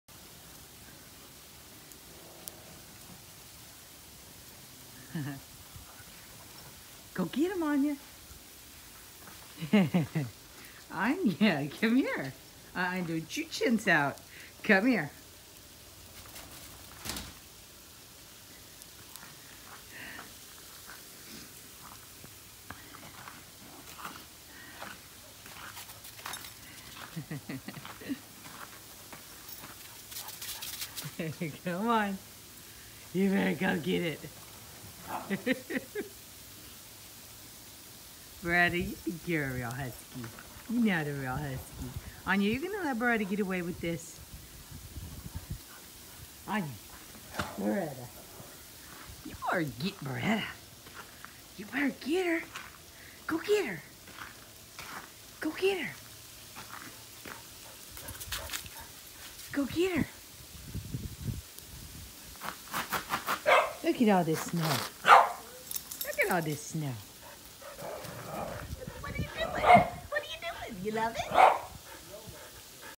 Huskies + Snow + Houston?
Snowing in Houston and Anya and Beretta love it!